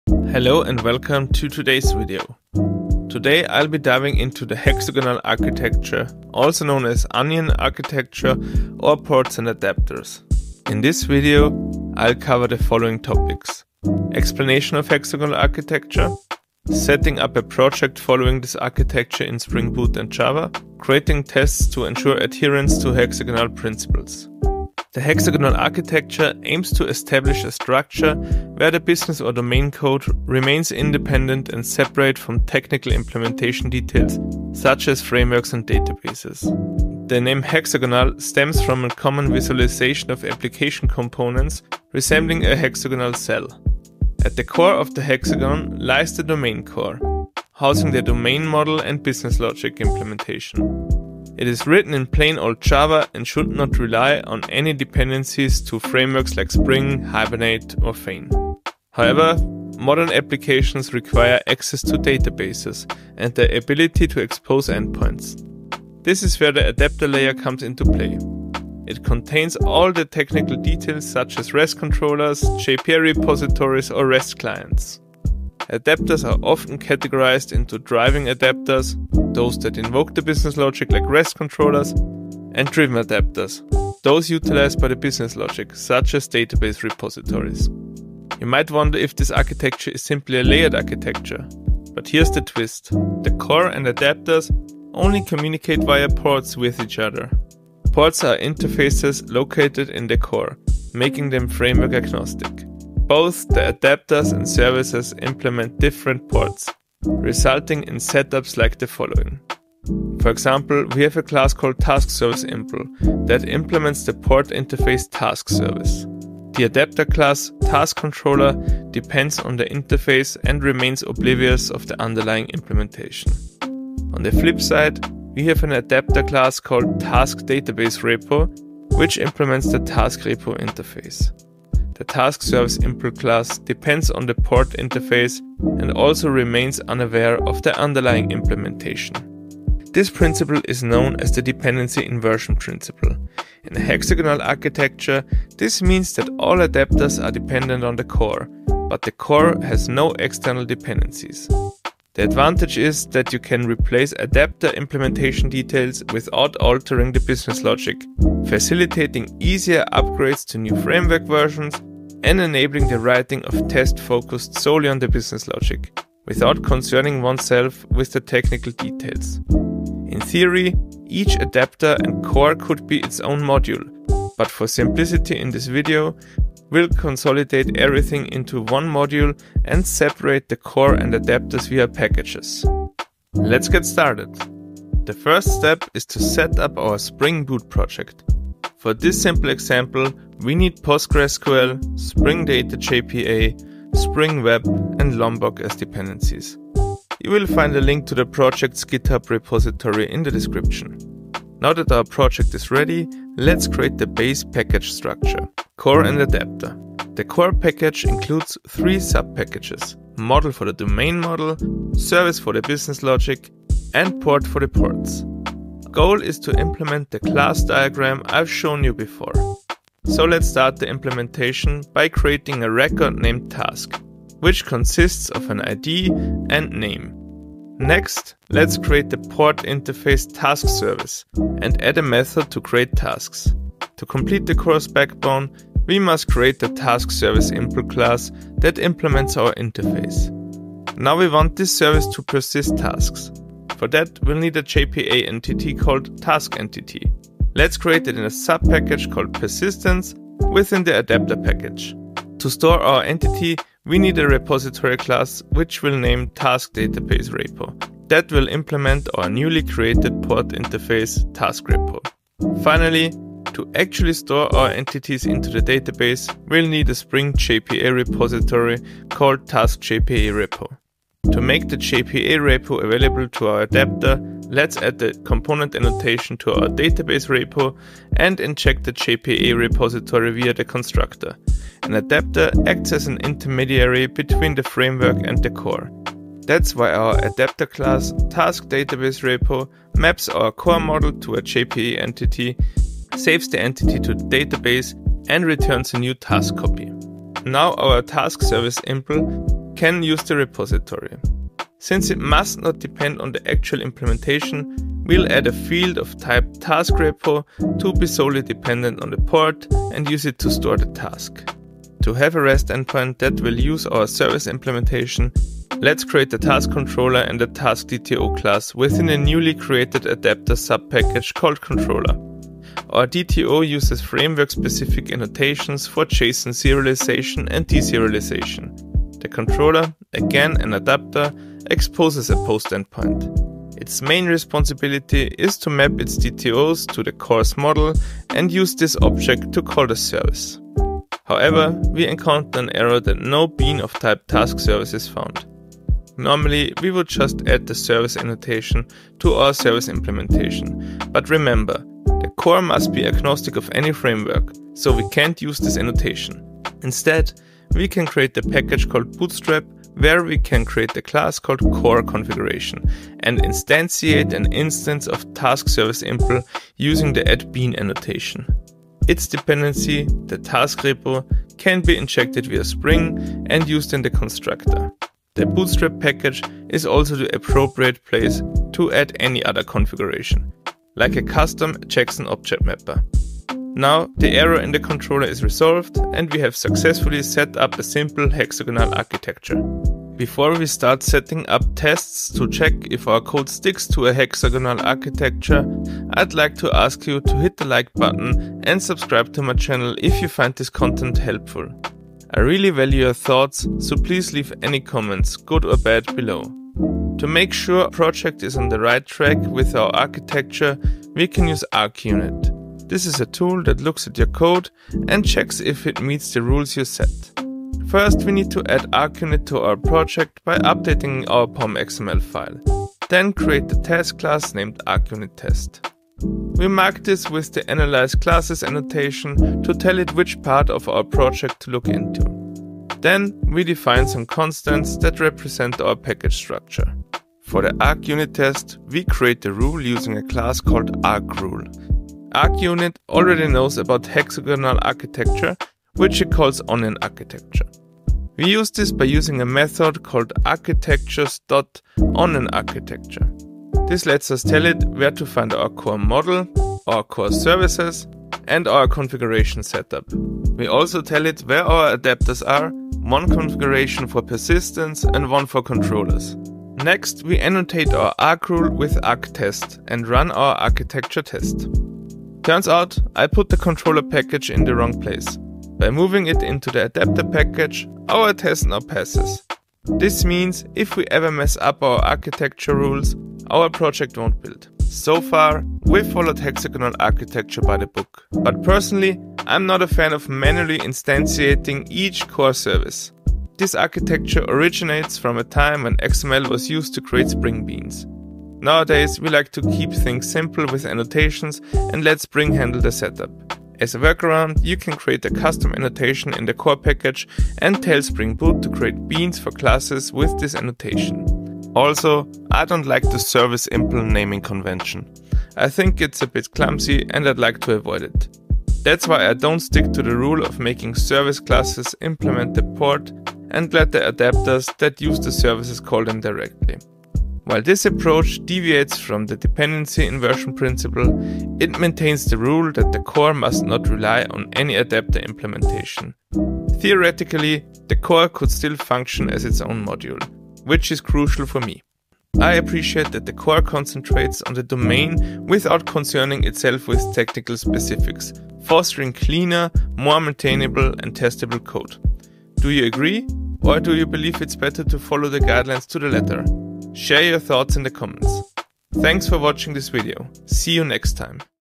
Setting up a Hexagonal Architecture in java and springboot (Onion Architecture, Ports&Adapters)
Welcome to our deep dive into Hexagonal Architecture, where we break down complex concepts into easy-to-understand explanations. If you're tired of getting lost in technical jargon and looking for a straightforward guide to improving your project's structure, you've come to the right place!

In this video, we cover everything from setting up your project to align with hexagonal architecture principles, using ArchUnit to ensure your code stays on track, to practical tips for making your code cleaner and more maintainable. Whether you're a seasoned developer or just starting out, you'll find valuable insights and tips to take your coding skills to the next level.

Your feedback matters to us, so please leave your thoughts, questions, or suggestions in the comments below.